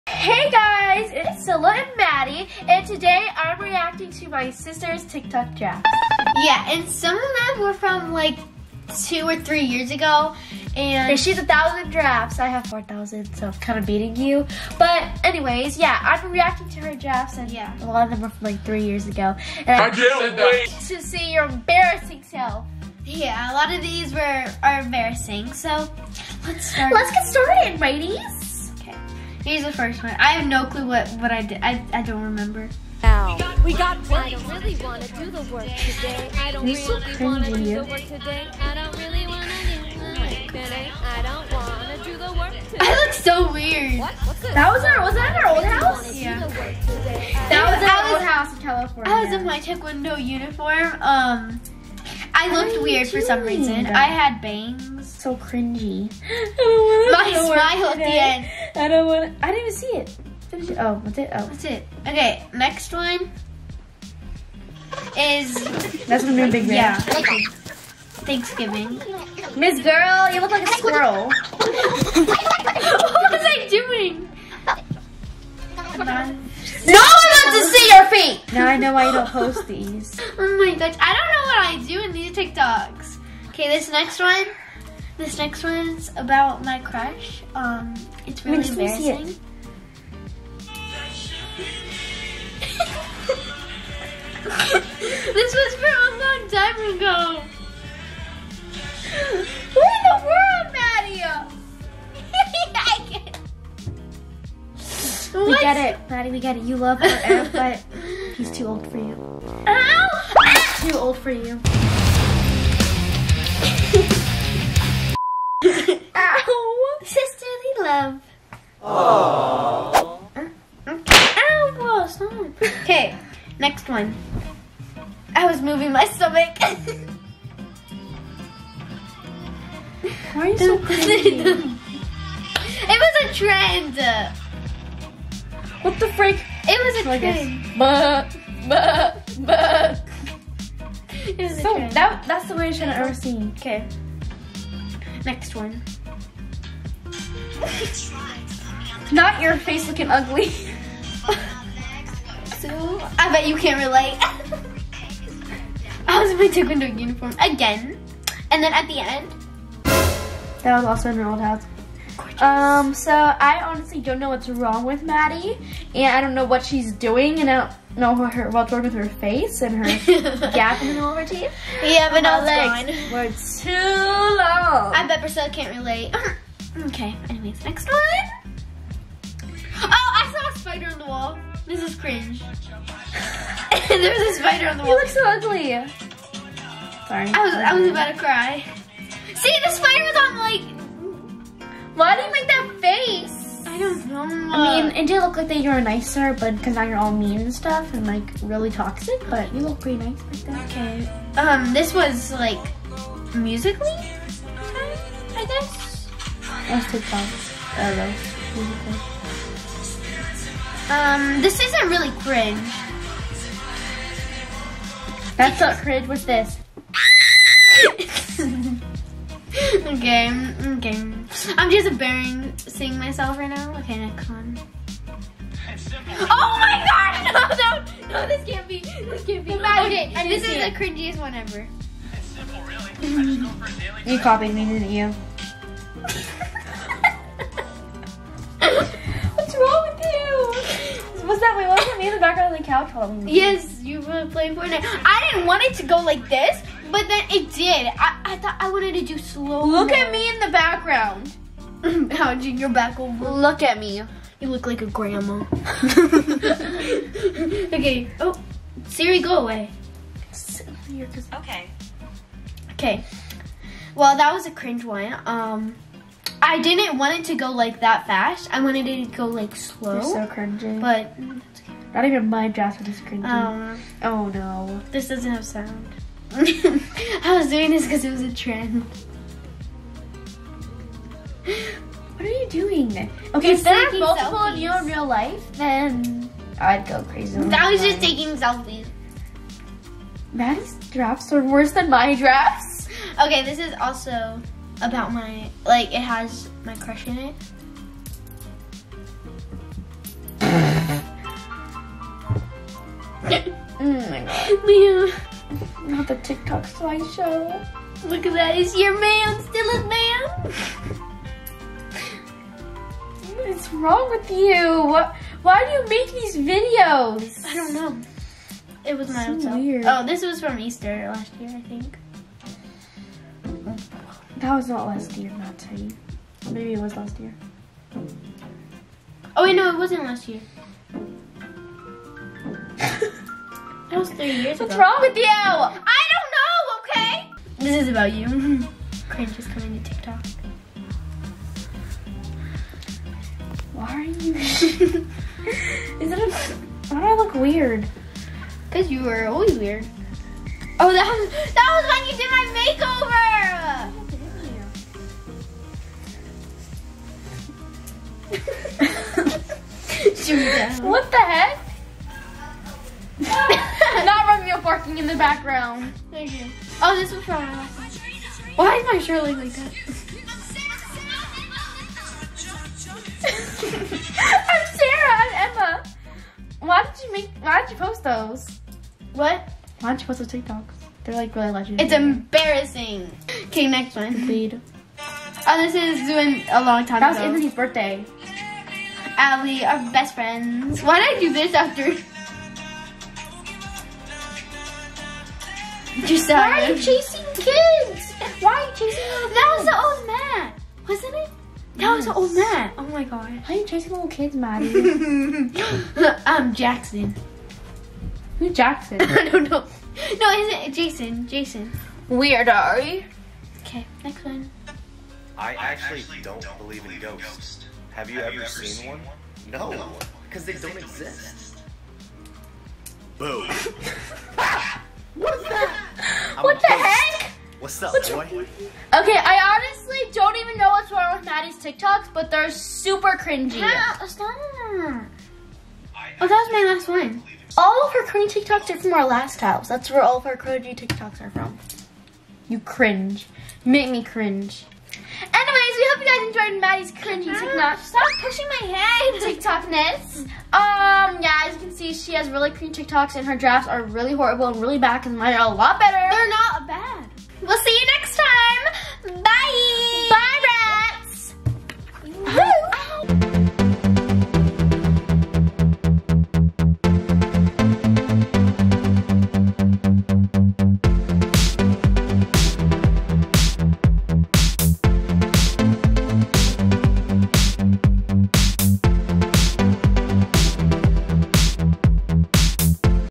REACTING TO MY LITTLE SISTER'S TIK TOKS DRAFTS!! **EXPOSED** | CILLA AND MADDY
Reacting to my little sister's tik toks drafts! Cilla is reacting to Maddy's Tik Toks drafts. Maddy has over 1000 drafts on her Tik Tok. She is too embarrassed to post them, so Cilla wanted to go through them. This was so funny! Maddy got exposed!!